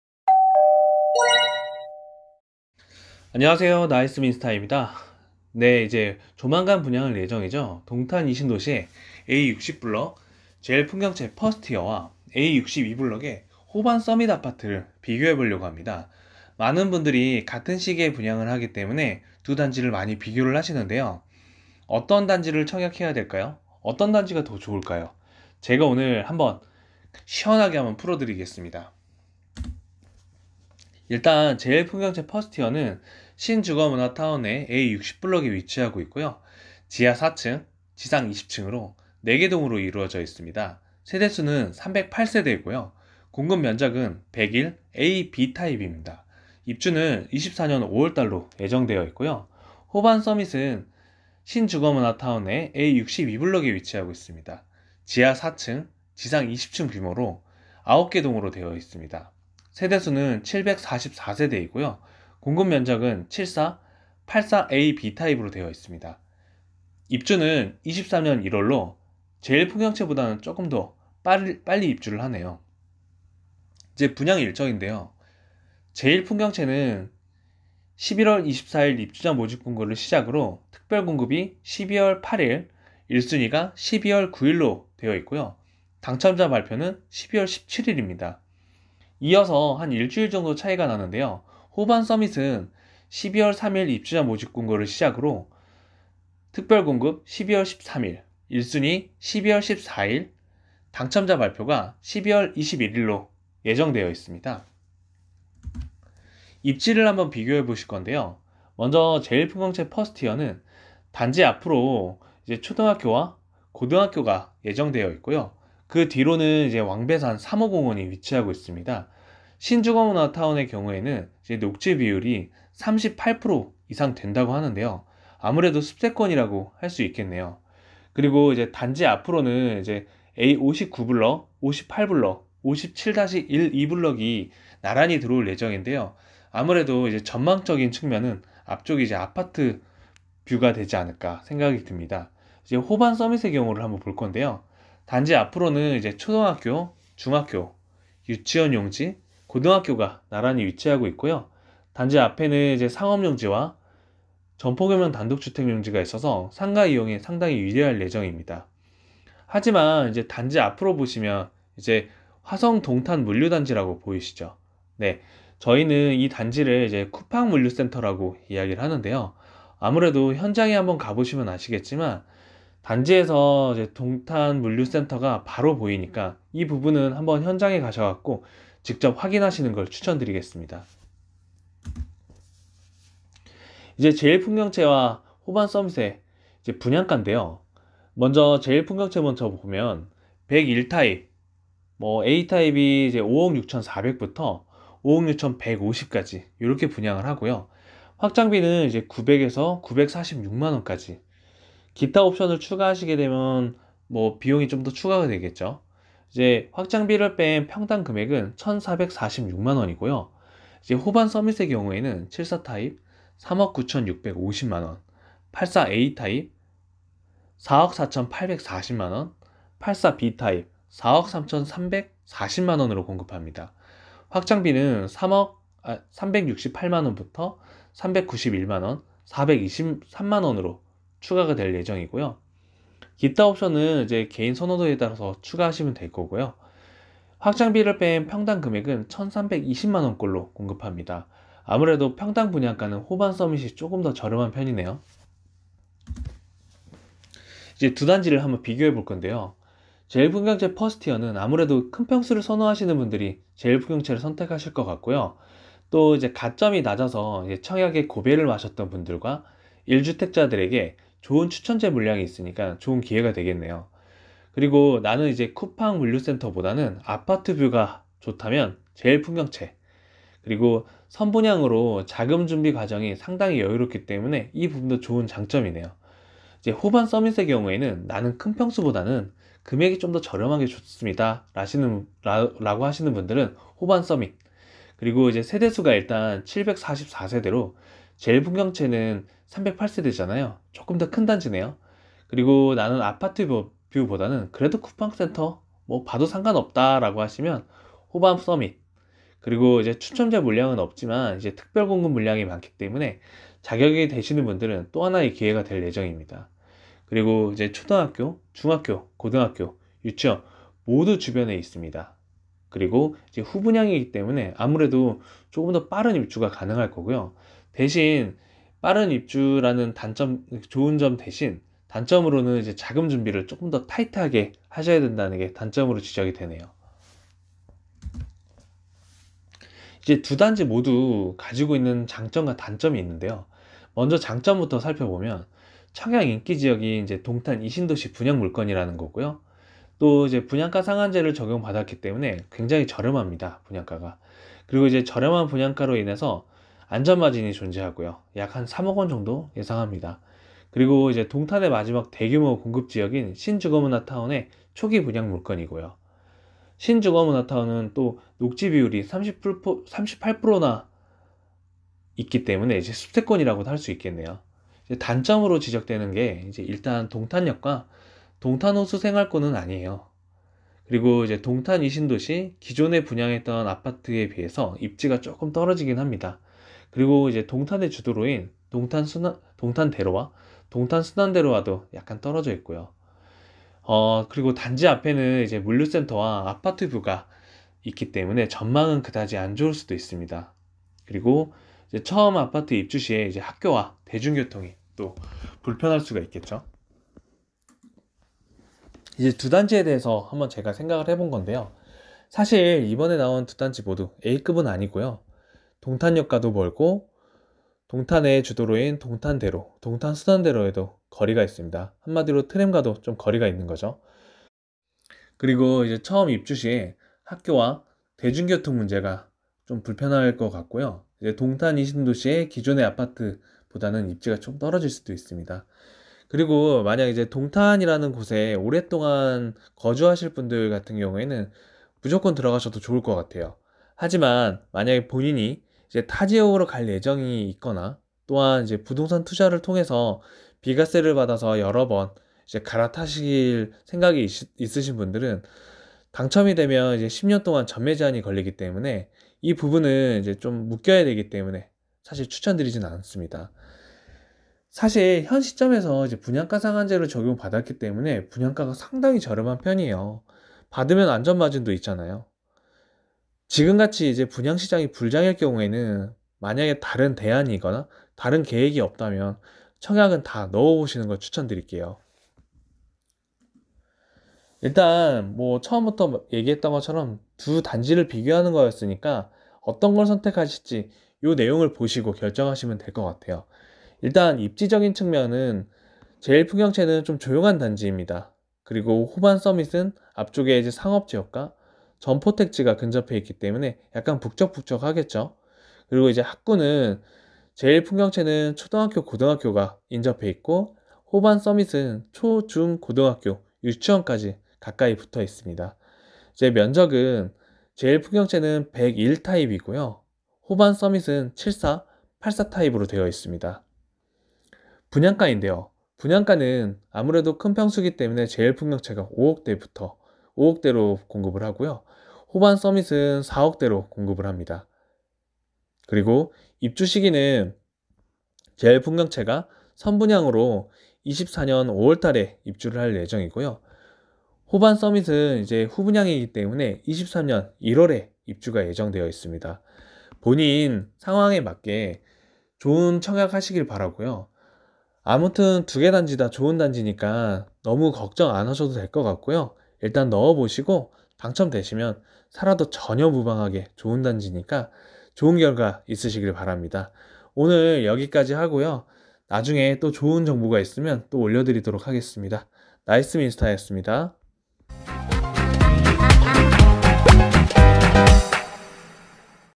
동탄2신도시 A60블럭 제일풍경채 vs A62블럭 호반써밋 신주거문화타운 신규분양단지 비교
동탄2신도시 신주거문화타운 신규분양단지 비교

A60블럭 제일풍경채 퍼스티어 VS A62블럭 호반써밋

어떤 단지가 좋을까요?

더 많은 정보를 보시려면 아래의 블로그에 방문해보세요. ^^